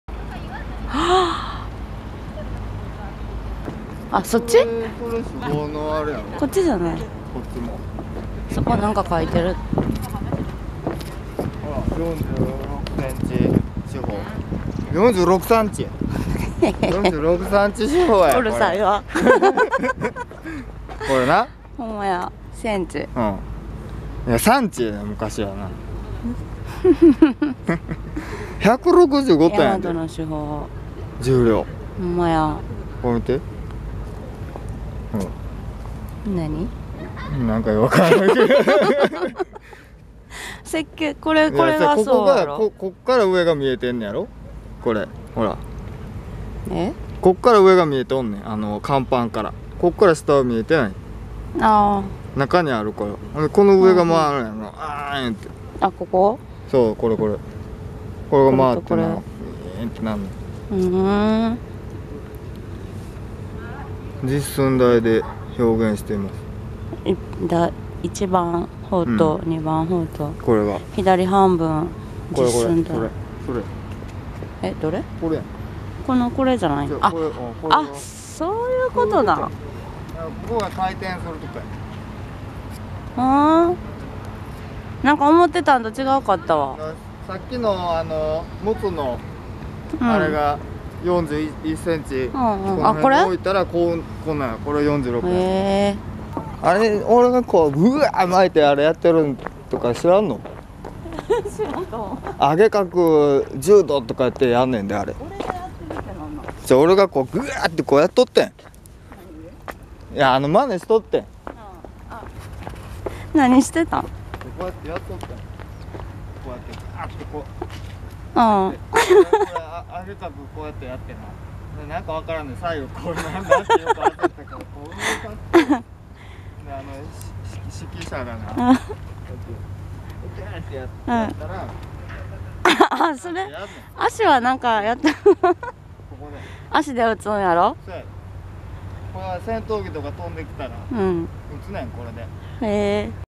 [広島旅行] 大和ミュージアムツアー 旅行 2021 12/29 アラフォーカップル
アラフォーカップル 大和ミュージアムツアー 旅行 2021 12/29
広島 呉 大和ミュージアム 2021.12/29
大和ミュージアム（呉市海事歴史科学館）
Evolution BR Racing  BuRaFu Racing エボリューションチャンネル